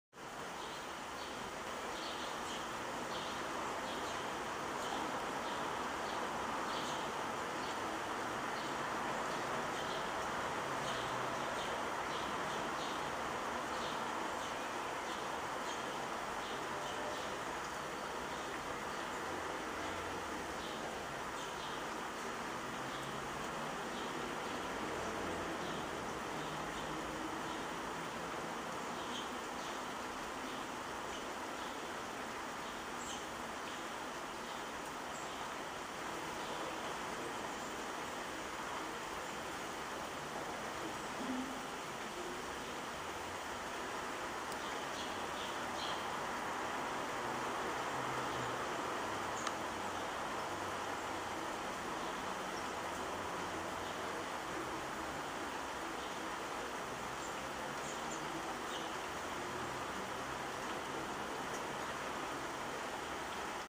Kohlmeise Parus Major
This Bird Likes To Eat From My Hand